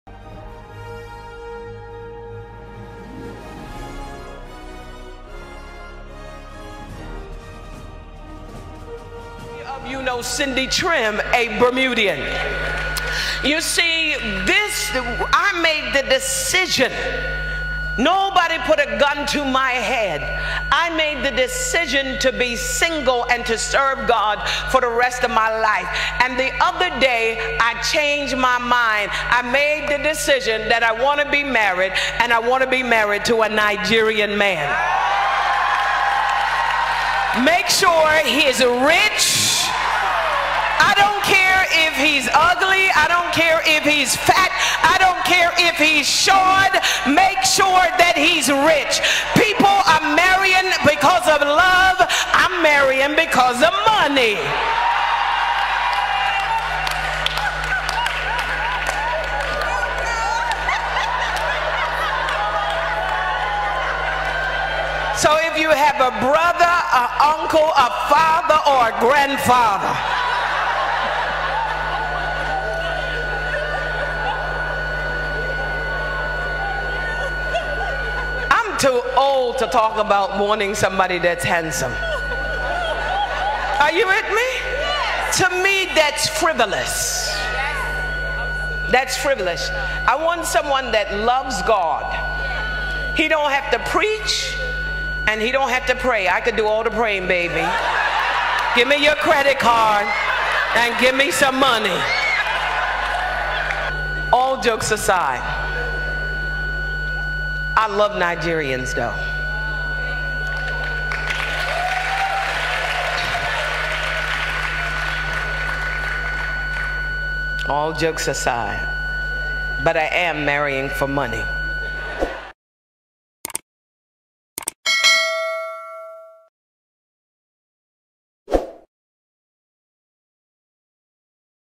"I want To Marry A Rich Nigerian, I Don't Care If He's Fat Or Short" - Black American Female Pastor
** APPEAL **